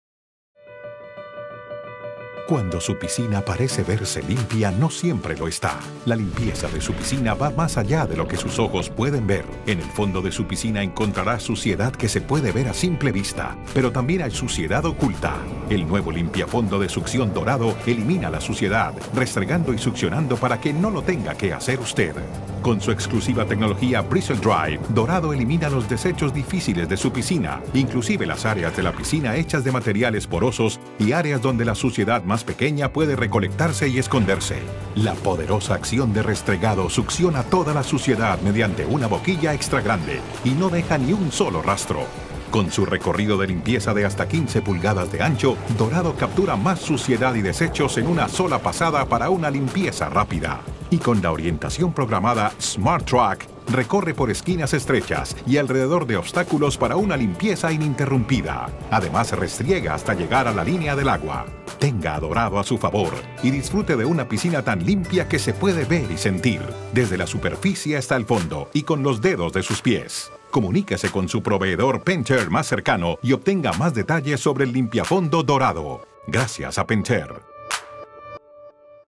Dorado - Limpiafondo de Succión
El limpiafondo de succión Dorado ofrece una limpieza simple y potente que se puede ver  y sentir.